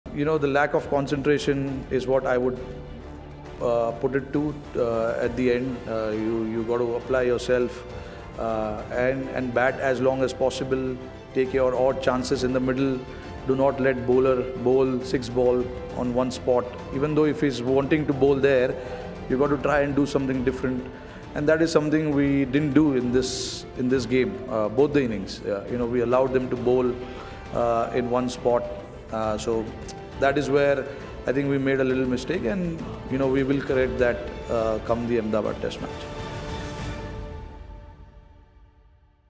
IND v AUS Test Series | Rohit Sharma on Team India’s Batting
🇮🇳 skipper Rohit Sharma talks about the batting lapse and TeamIndia making amends & coming back harder next game. 💪🏏

Tune-in to the 4th Mastercard INDvAUS Test
Thursday | 8:30 AM onwards | Star Sports Network & Disney+Hotstar